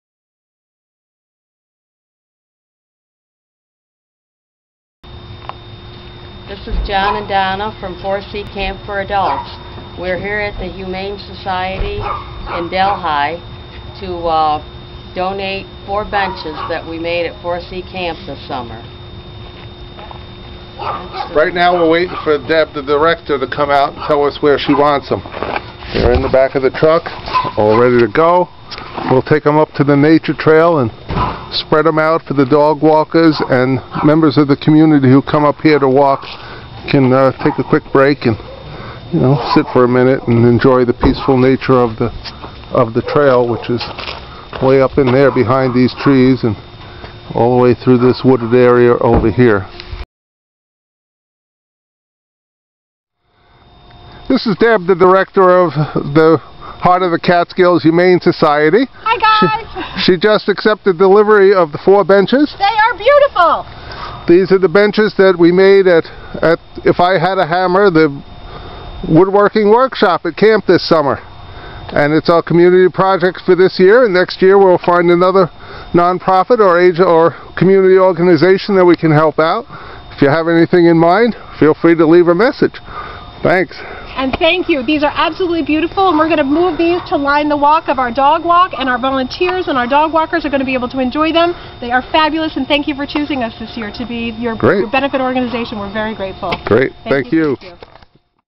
Heart of the Catskills Humane Society Receives New Benches
Participants in 4C Camp for Adults 2014 constructed four benches in their woodworking workshop and donated them to HCHS to line the Nature Trail behind the shelter for dog-walkers and volunteers to enjoy.